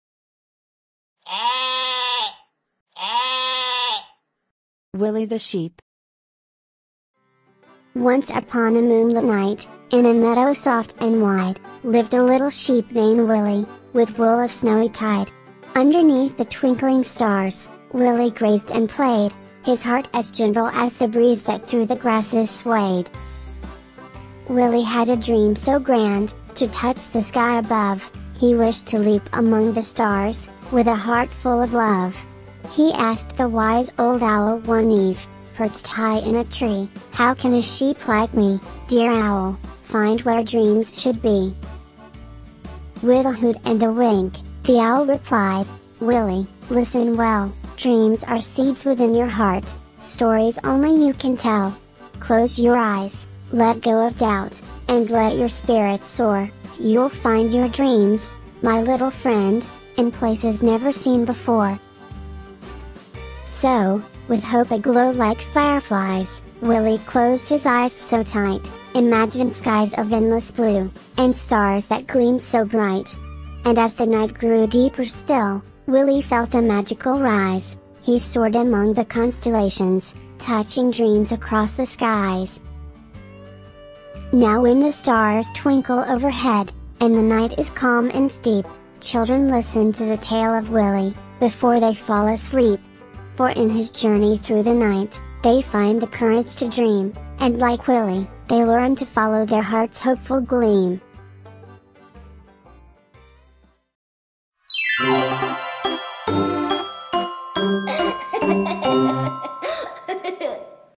Willy The Sheep - Old Sally's Tales
Join Willy the sheep on a moonlit adventure through a meadow of dreams in this charming bedtime story. With a heart as gentle as the breeze, Willy yearns to touch the stars and seeks guidance from a wise old owl. Through courage and imagination, Willy discovers the magic of following his heart's desires, inspiring young readers to do the same.